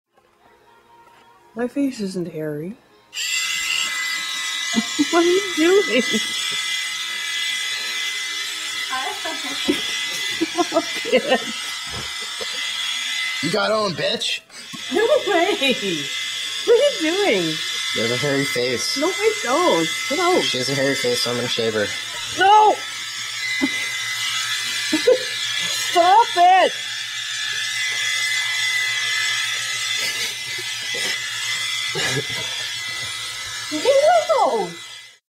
A good shave
Tyler messing with his mom. Surprised it didn't catch them side burns. She's a cool mom.